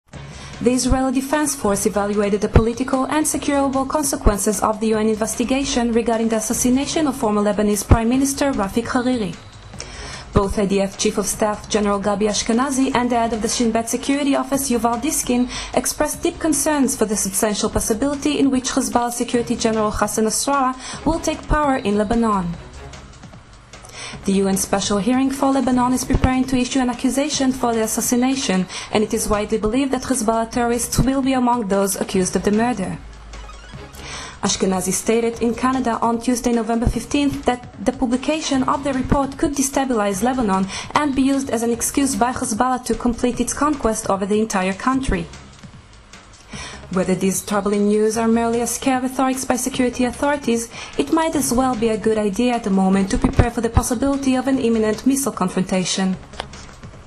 Nasralla will take power in Lebanon ?
The Israeli Defense Force evaluated the political and securable consequences of the UN investigation regarding the assassination of former Lebanese Prime Minister Rafik Hariri.
Both IDf chief of staff- Gen. Gabi Ashkenazi and the head of the Shin Bet security office- Yuval Diskin expressed deep concerns for the substantial possibility in which Hezbollah Security General Hassan Nasralla will take power in Lebanon.
The U.N. Special hearing for Lebanon is preparing to issue an accusation for the assassination, and it is widely believed that Hezbollah terrorists will be among those accused of the murder.
Ashkenazi stated in Canada on Tuesday November 15th that "The publication of the report could destabilize Lebanon and be used as an excuse by Hezbollah to complete its conquest over the entire country."
Weather these troubling news are merely a scare rhetoric by security authorities, it might as well be a good Idea at the moment to prepare for the possibility of an imminent missile confrontation.